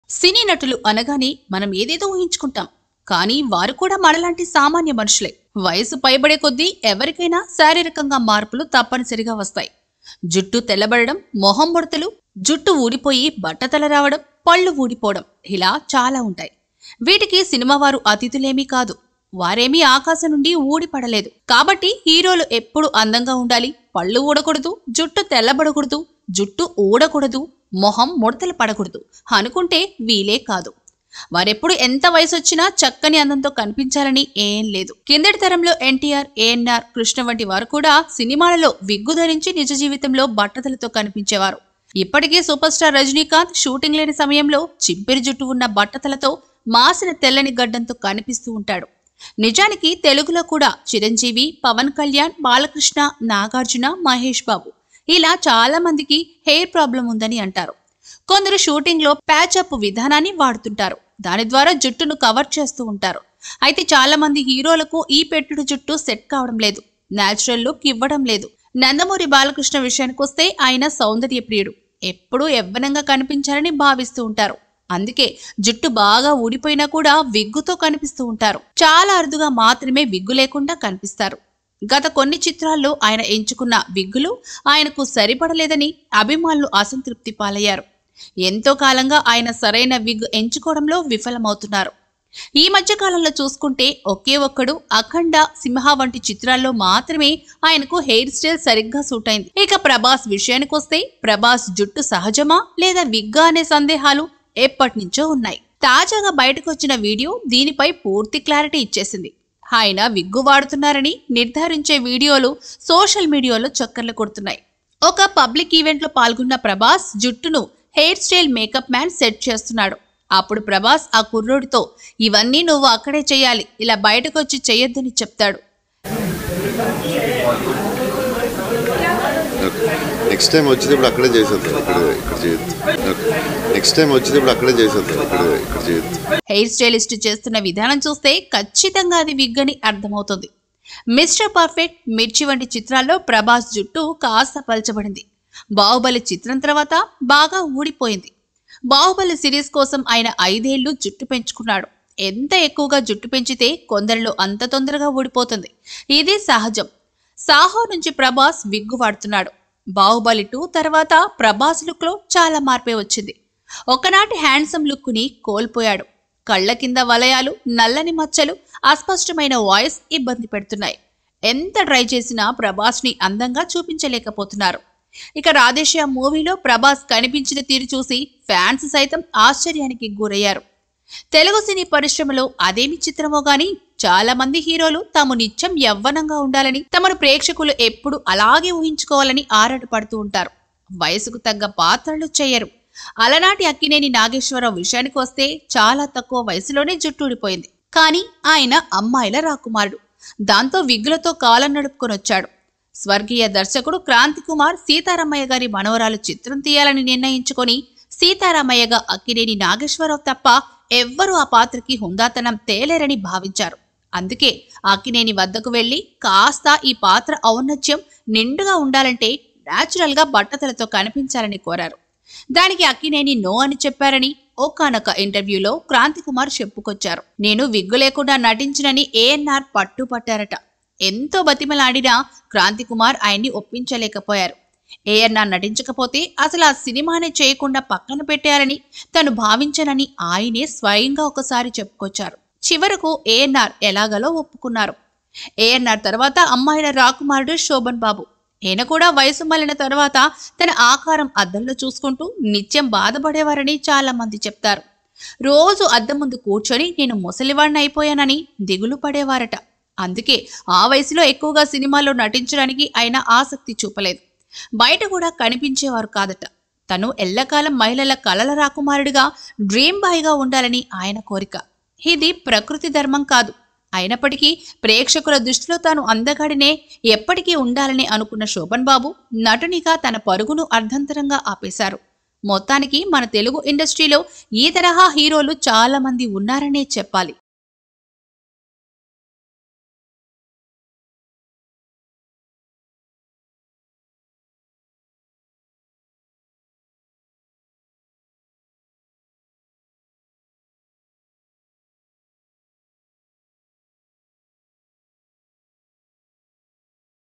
Adipurush Prabhas Using A Wig? | ప్రభాస్ విగ్ చూసి షాక్ అవుతున్న అభిమానులు..!! TeluguOne
Adipurush, Salaar actor Prabhas use a wig? Reason Behind Prabhas having Wig.